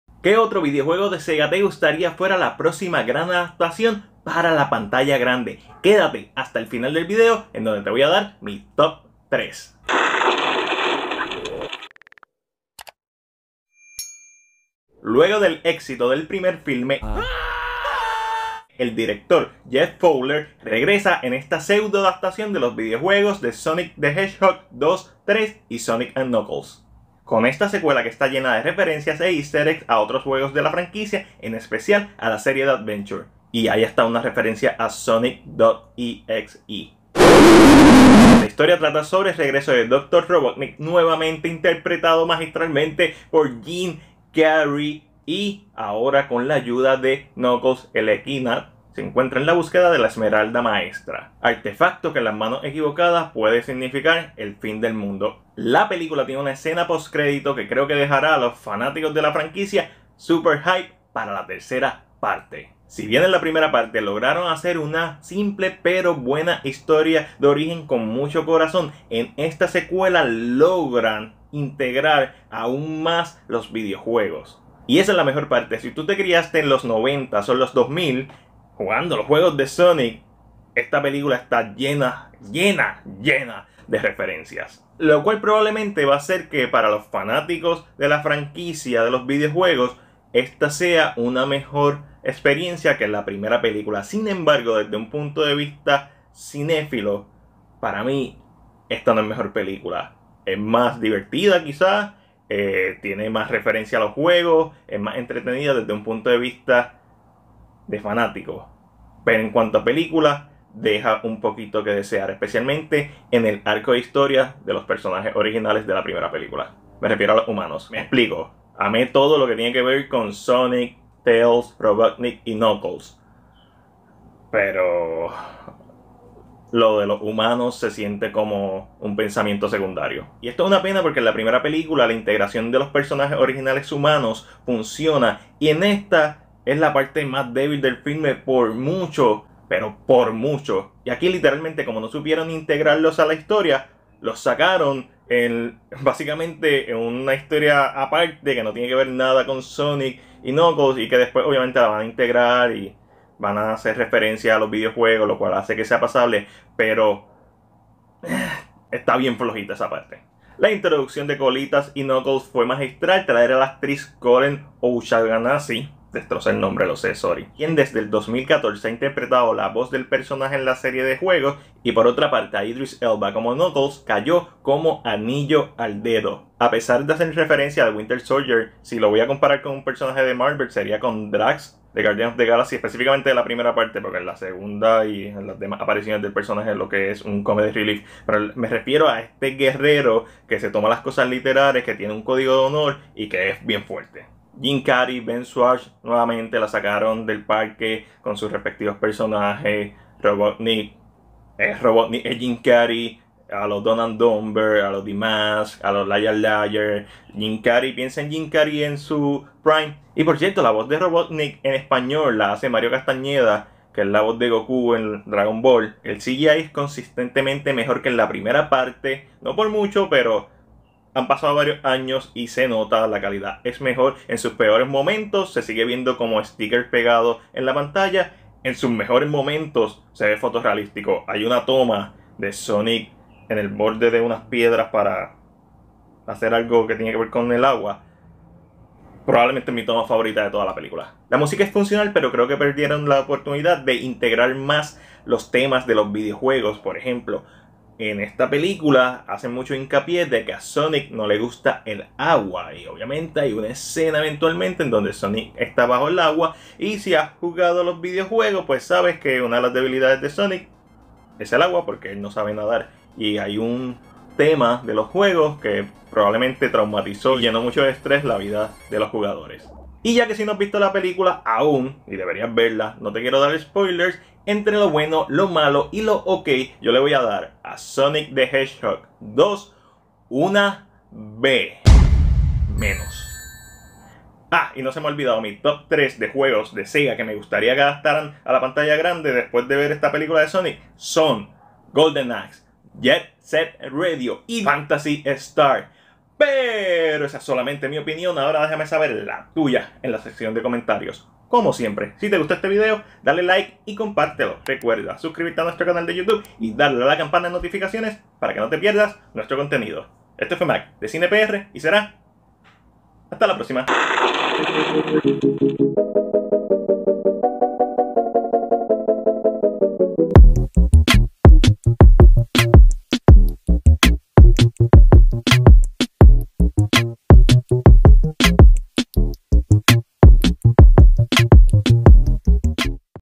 Sonic 2 (2022) Reseña
Sonic y compañía regresan a la pantalla grande para esta secuela llena de referencias y guiños de los video juegos.